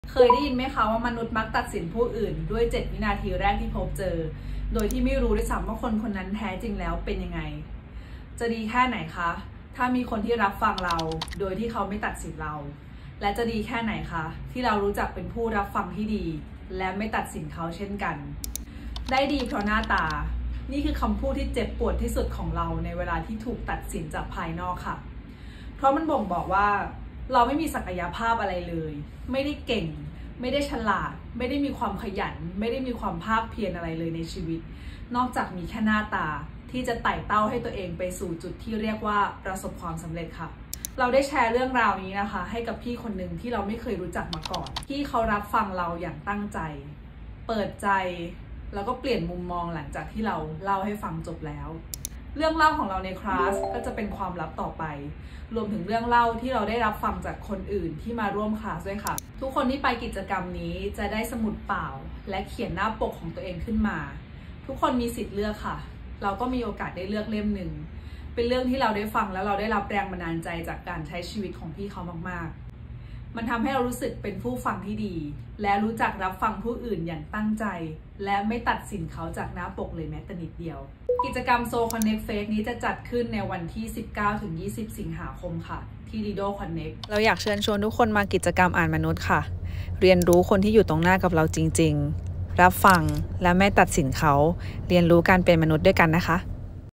มนุษย์มักตัดสินใจคนอื่น?
มนุษย์มักตัดสินคนอื่น จริงมั้ยคะ? 🙂
📍กิจกรรม “อ่านมนุษย์”

สำหรับใครที่สนใจกิจกรรมดี ๆ ที่ช่วยฮีลใจดี ๆ แบบนี้ สสส. และภาคีเครือข่ายเขาเปิดโอกาสให้เราได้ไปสัมผัสด้วยตัวเองกันแบบฟรี ๆ ไม่มีค่าใช้จ่ายที่งาน Soul Connect Fest 2023 มหกรรมพบเพื่อนใจ งานที่จะพาเราออกเดินทางค้นหาความสุข ฮีลใจไปด้วยกันผ่านเครื่องมือและกิจกรรมที่หลากหลาย เช่น นิทรรศการ เวิร์คชอป การแสดงดนตรี ฯลฯ วันที่ 19-20 สิงหาคม 66 ที่ Lido Connect สยามสแควร์
งานนี้เขาเปิดให้ลงทะเบียนเข้าร่วมกิจกรรมแล้วที่
หมายเหตุ : บางกิจกรรมรับจำนวนจำกัด รีบกันหน่อยนะ

**Soul Connect Fest จัดโดยภาคีเครือข่ายด้านสุขภาวะทางปัญญามากกว่า 50 องค์กร ภายใต้การสนับสนุนของสำนักสร้างเสริมระบบสื่อและสุขภาวะทางปัญญา สสส.